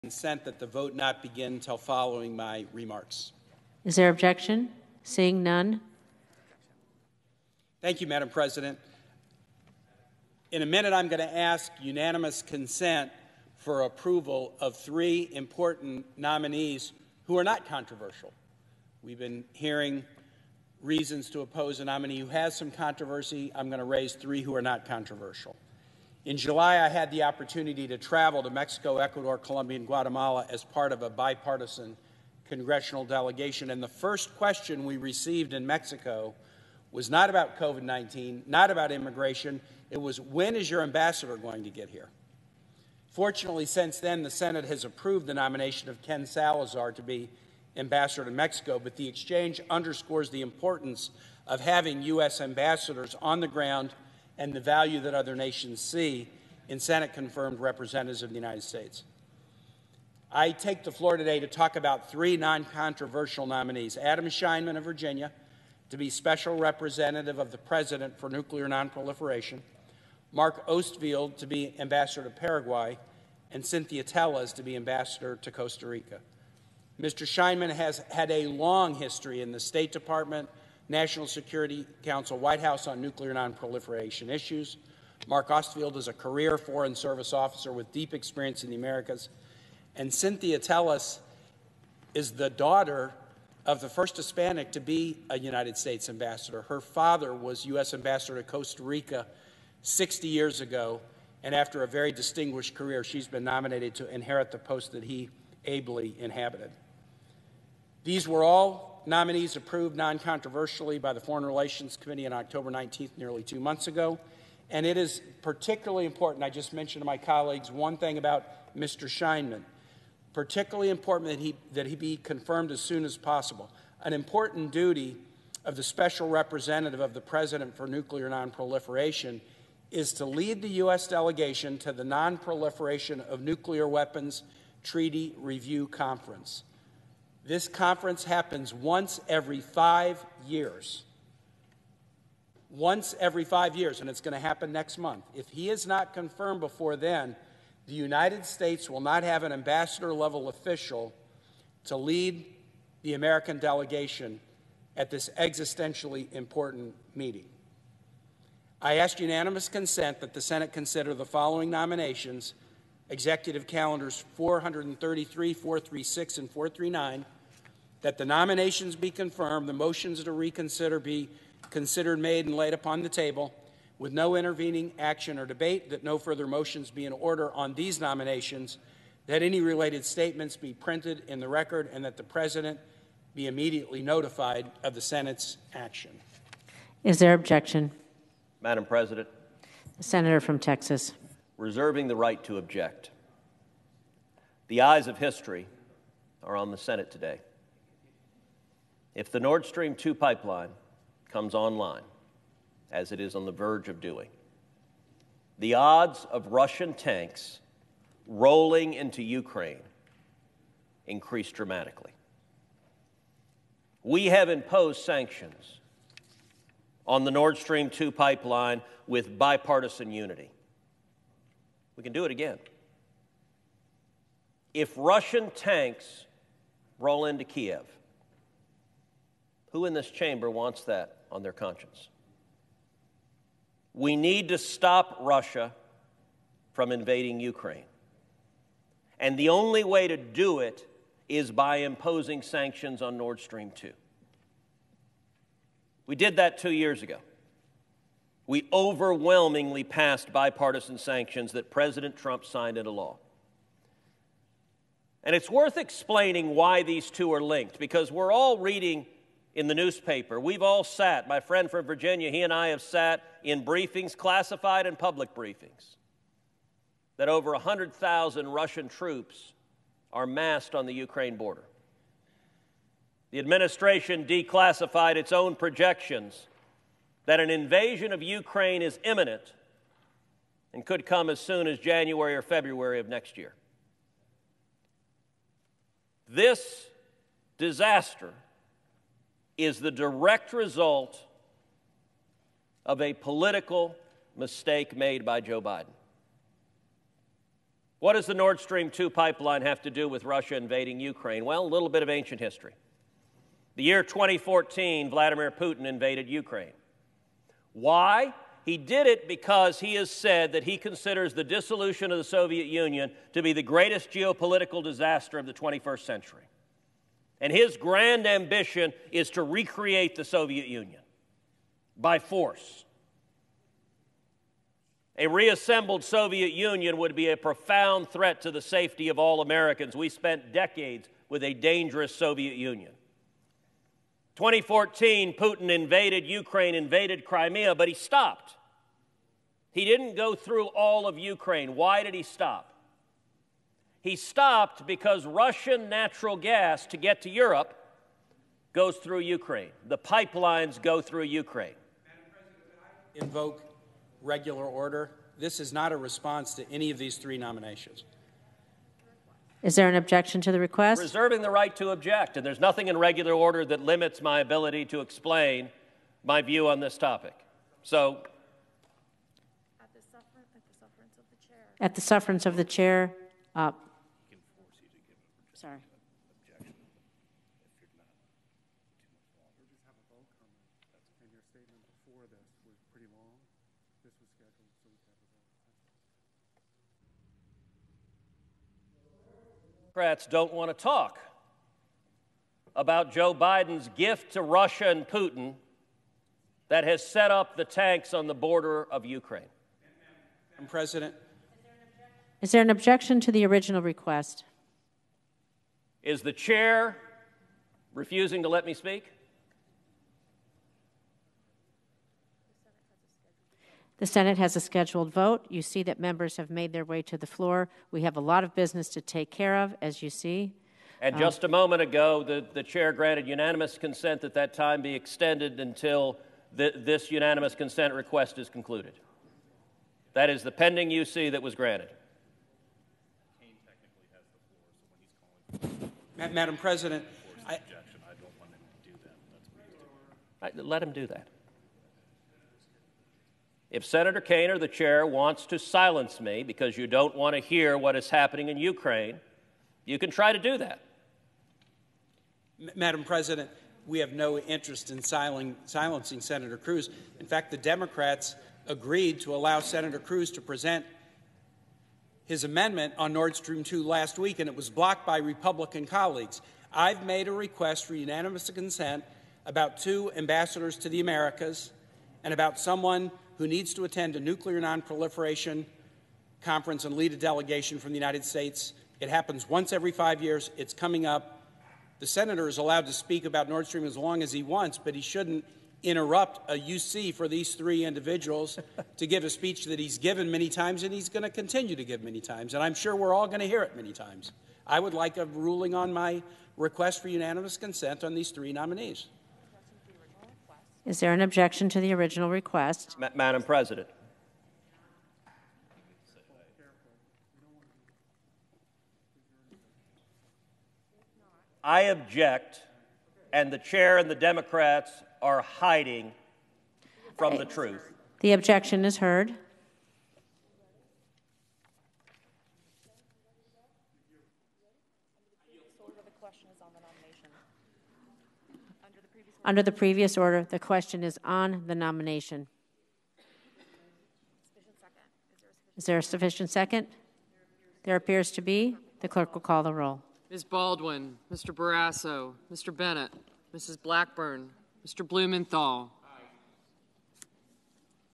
JUST IN: Ted Cruz, Tim Kaine Share Tense Senate Floor Exchange: 'Hiding From The Truth'
Sen. Ted Cruz (R-TX) attempts to block key nominees over Nord Stream 2 concerns, which is pushed back by Sen. Tim Kaine (D-VA).